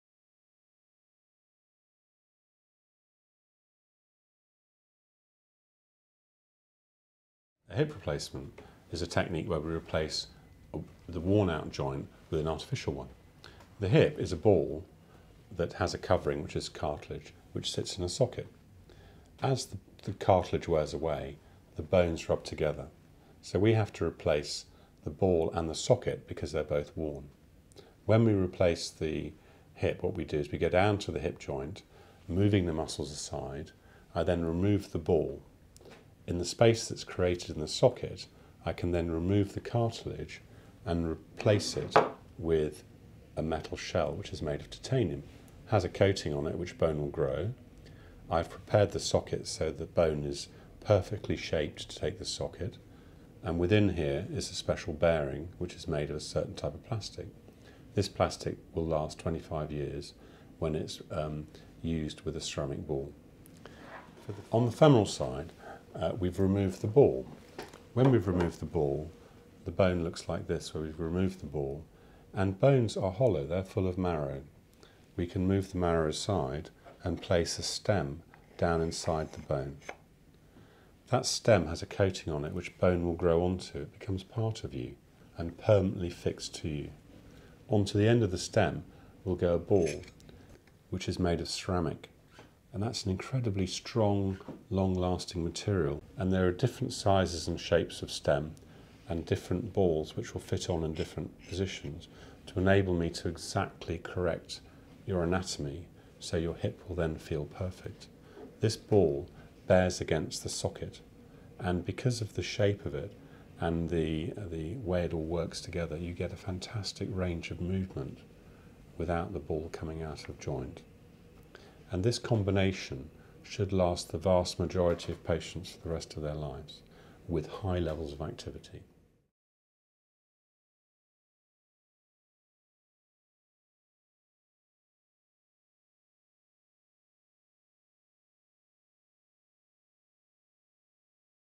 An introduction to a hip replacement
Mr Hugh Apthorp goes through the steps of a hip replacement and the constituent parts that make up a hip prosthesis (artificial hip).